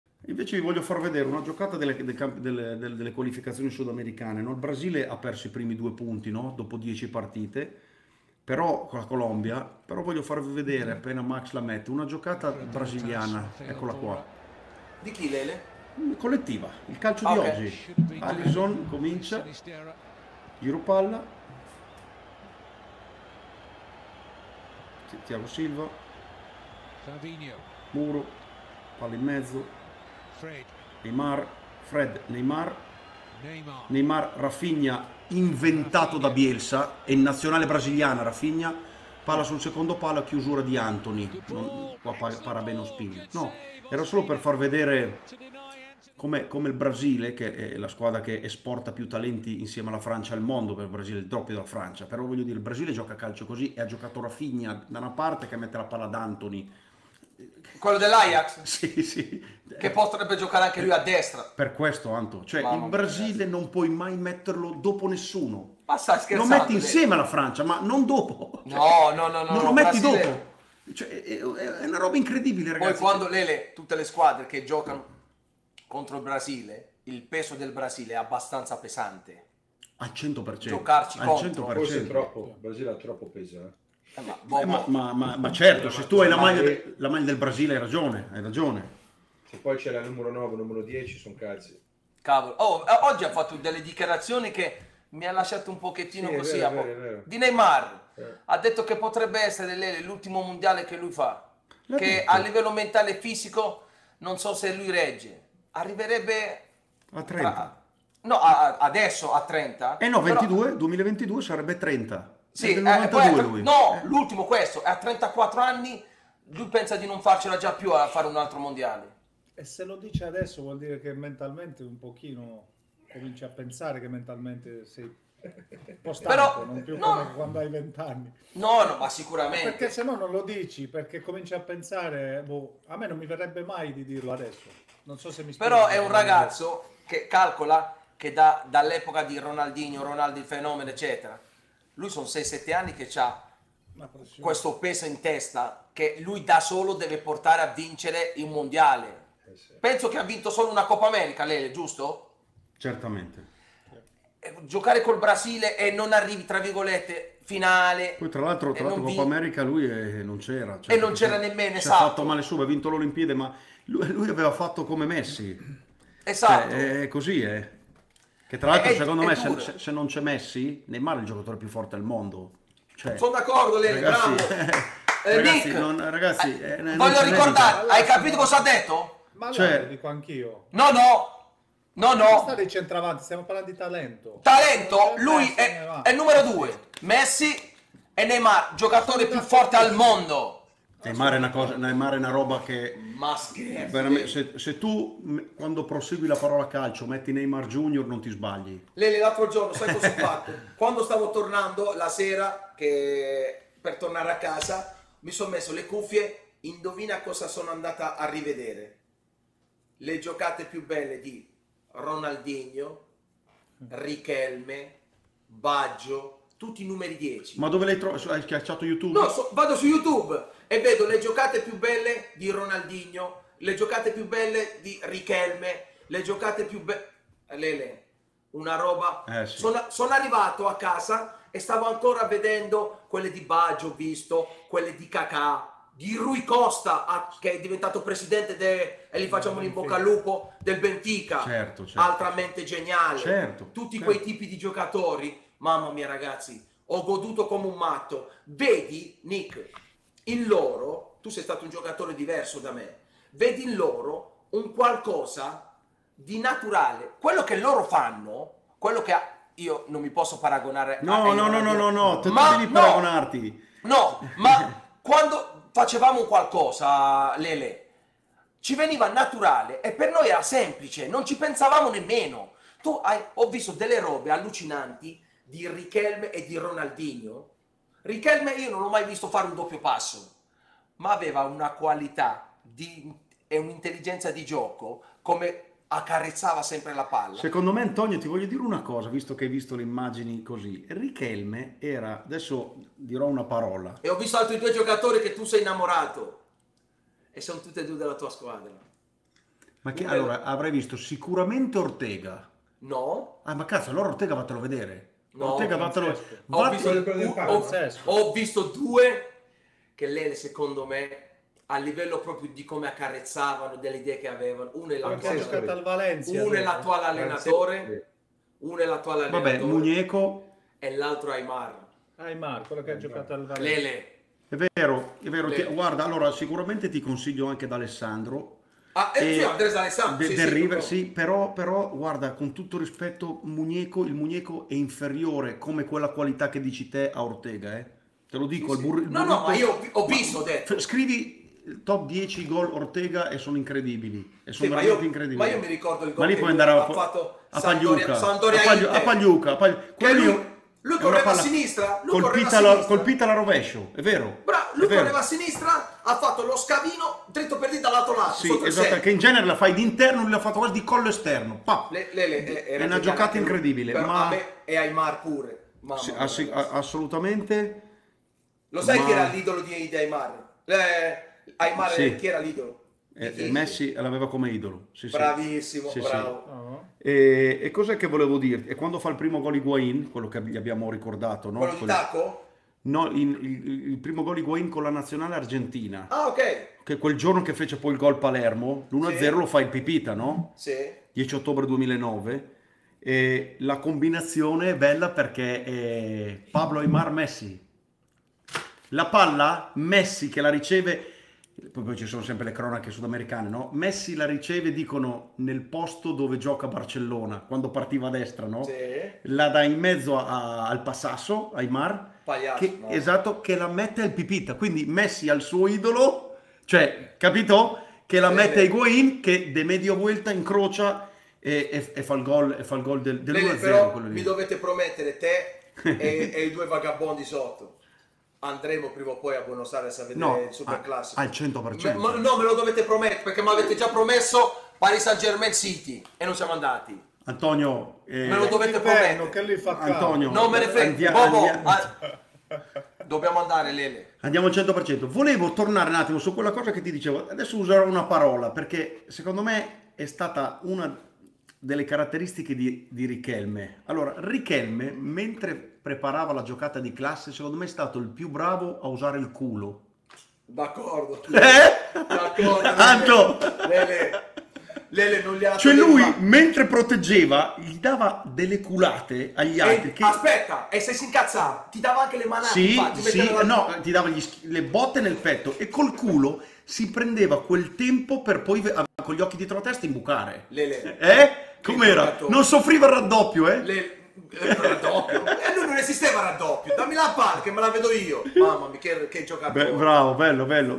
BOBO TV - ADANI: "Se non c'è Messi, Neymar è il giocatore più forte al mondo"
Alla Bobo Tv i 4 Bomber Vieri, Adani, Ventola e Cassano parlano delle qualificazioni mondiali del Sudamerica e delle leggende di Brasile e Argentina

Sei d'accordo con i bomber?
Commenta e facci sapere la tua.